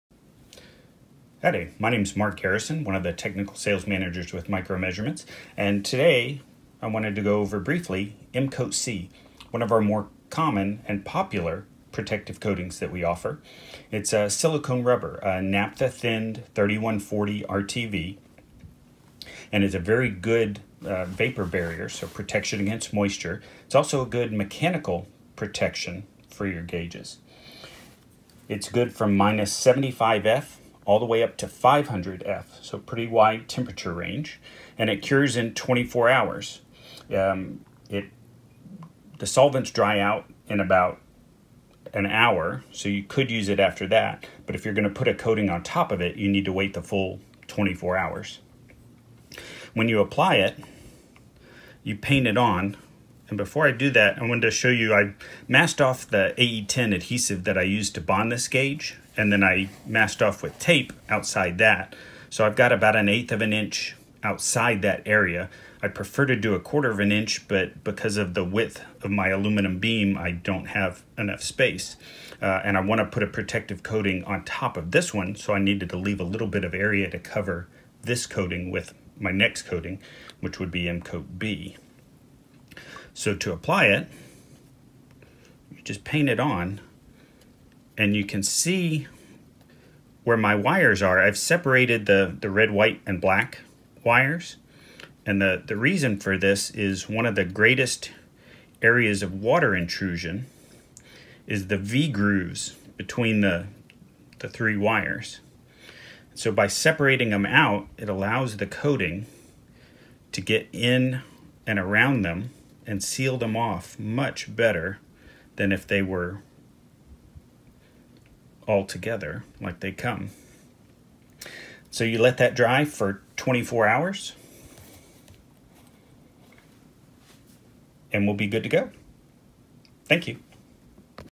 Protective Coating for Strain Gage Stability. (M-Coat C)
Mark discusses the merits of one of the most commonly used strain gage protective coatings; M-Coat C.
Solvent-thinned (naphtha) RTV silicone rubber. Cures to tough, rubbery transparent film.
Good all-around mechanical and electrical properties. Completely noncorrosive.
Film thickness 0.015–0.02 in (0.4–0.5 mm) per coat.
Recommended for lab and field installations that require a high degree of protection in thin coatings. Good water splash protection. Good chemical resistance.